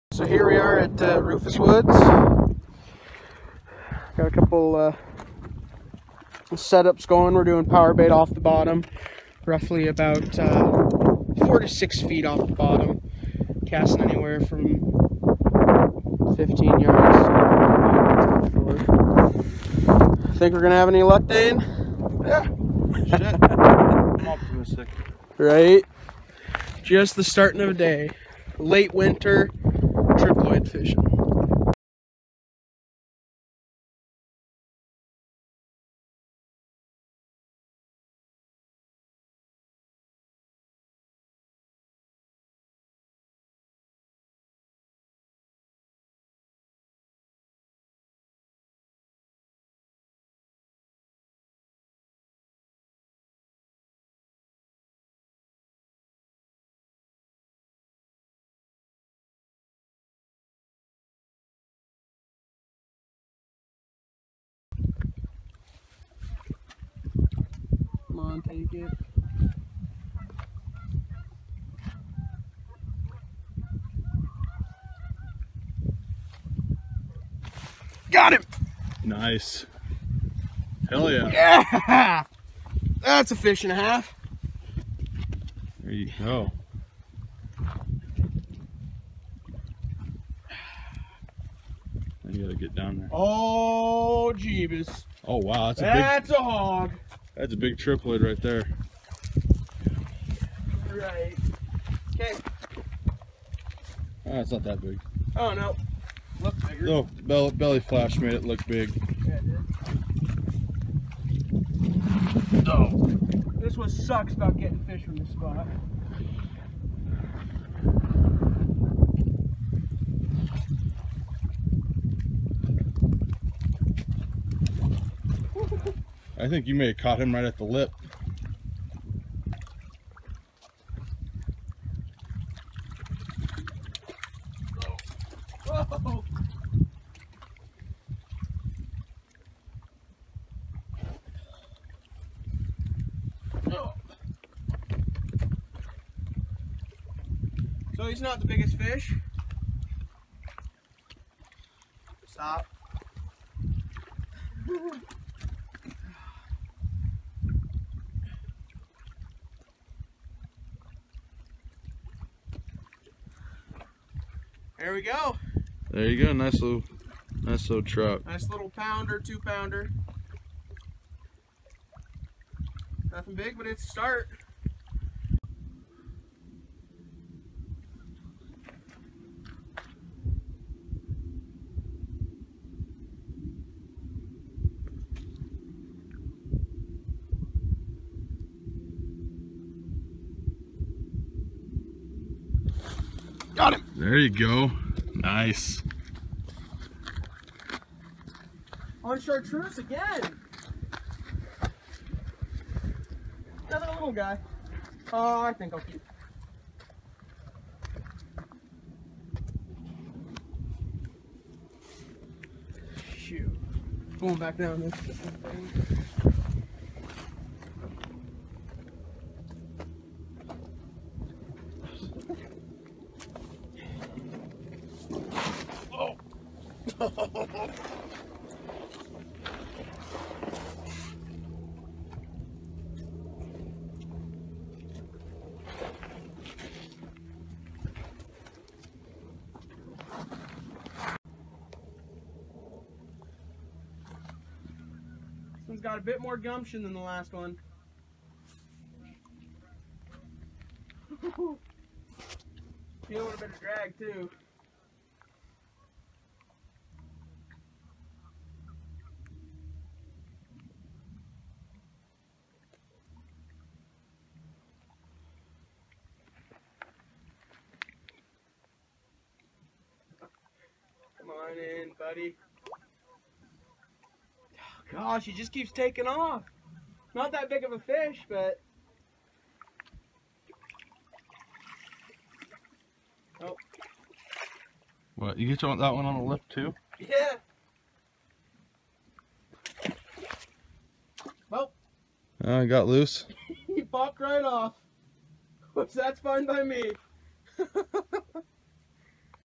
Roofus Woods Triploid Fishing
Late winter fishing for some Triploids up Roofus Woods which is the section of the Columbia River upstream behind Chief Joseph Dam.  This was a slow day for sure but we got two triploids.  Chartreuse was the color of choice.   We where using Power Bait with leader everywhere from four feet to six feet.  Enjoy folks!